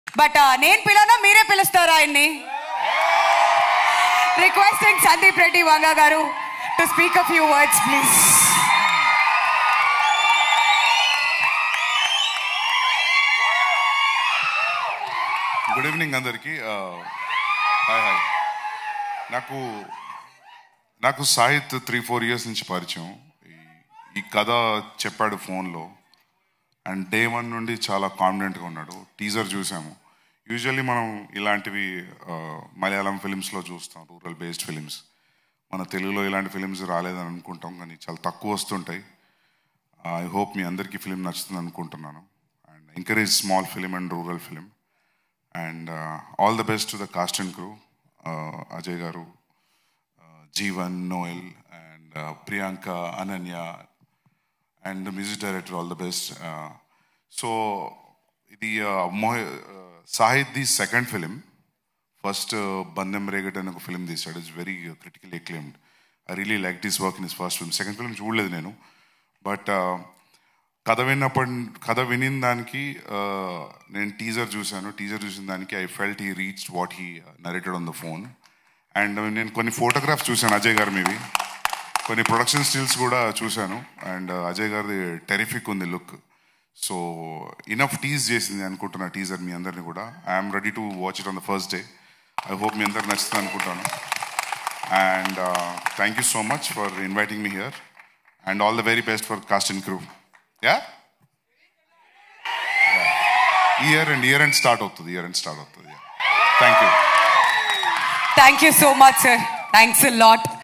Sandeep Reddy Vanga Speech At Pottel Teaser Launch Event | Ananya Nagalla | Santosham Updates
WATCH : Sandeep Reddy Vanga Speech At Pottel Teaser Launch Event | Ananya Nagalla | Santosham Updates